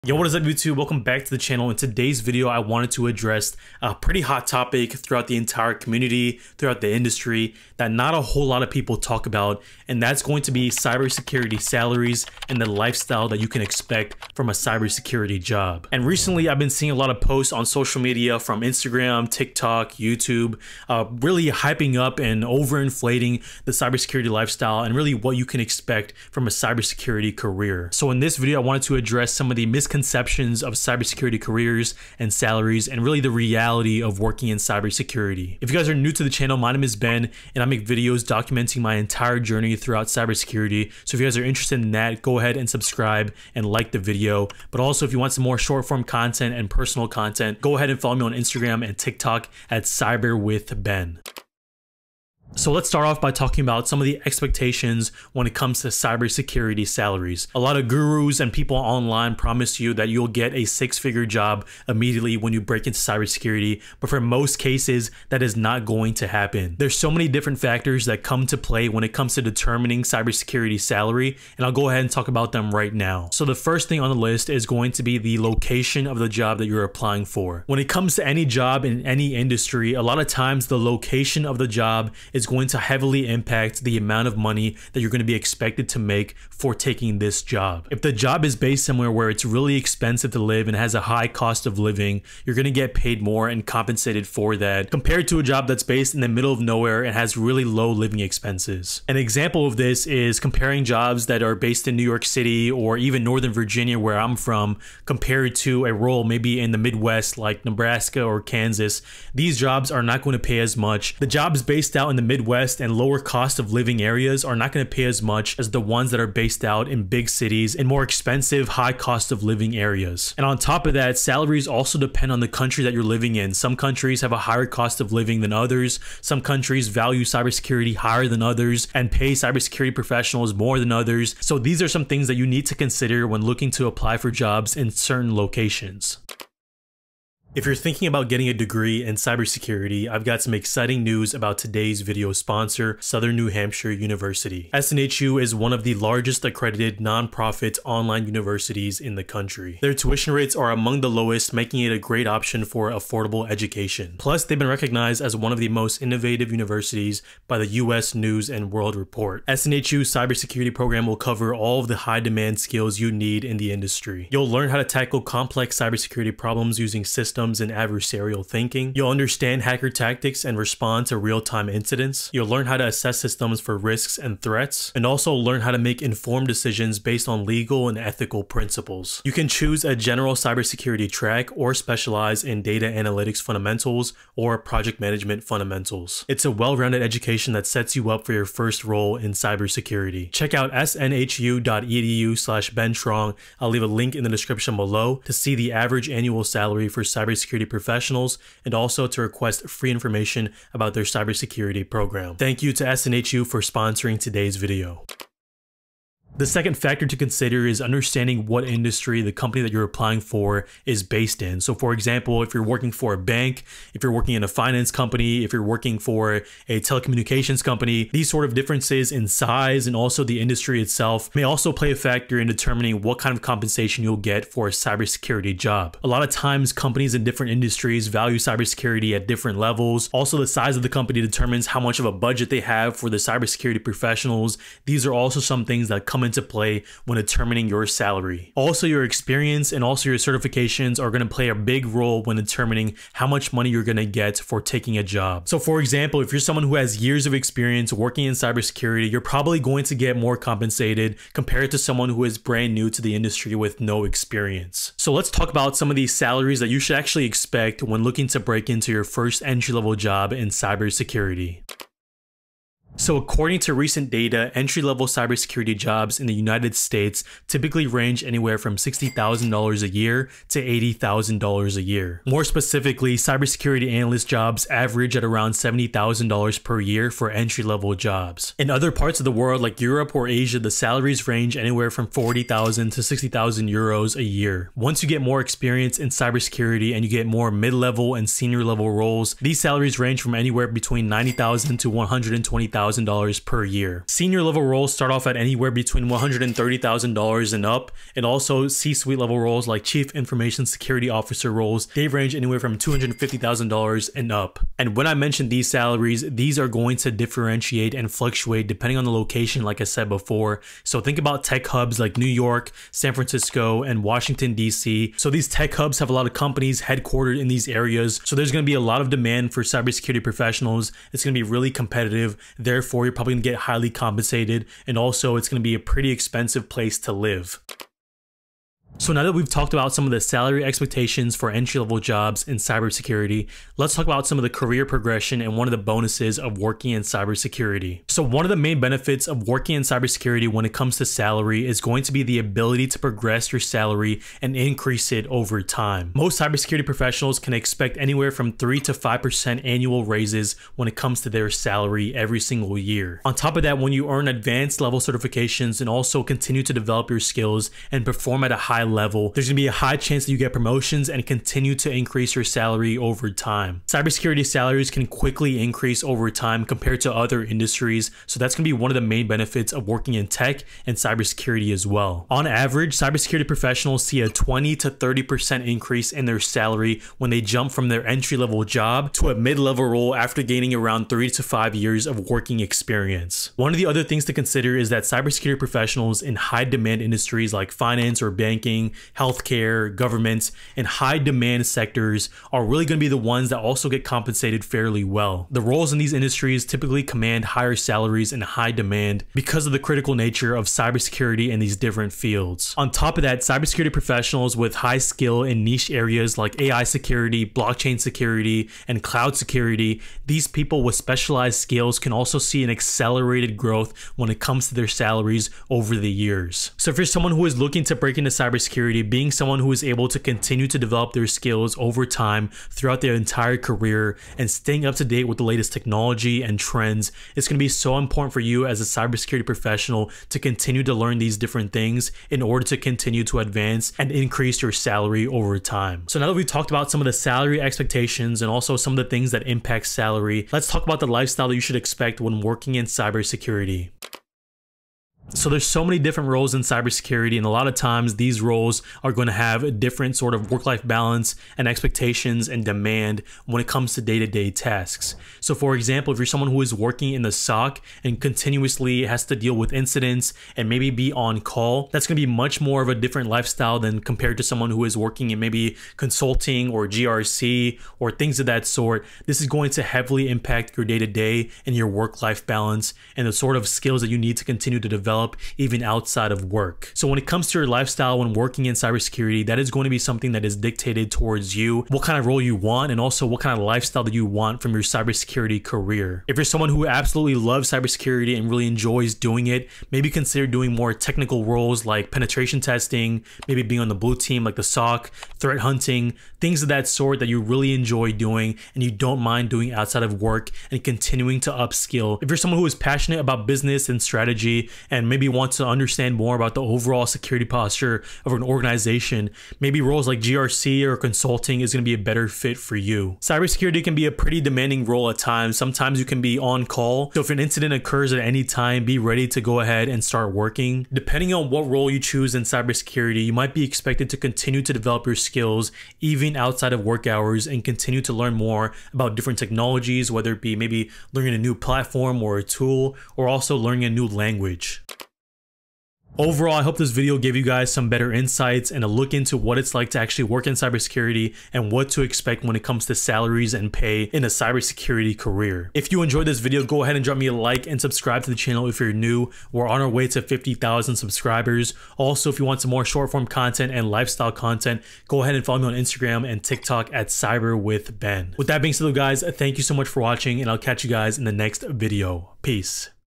The Truth About Cyber Security Salaries and Lifestyle
In this video, I speak about some of the misconceptions that beginners have when looking to land their first role in cybersecurity. We go over cyber security salaries, lifestyle, and more!

✍️ Start using Notion for notes!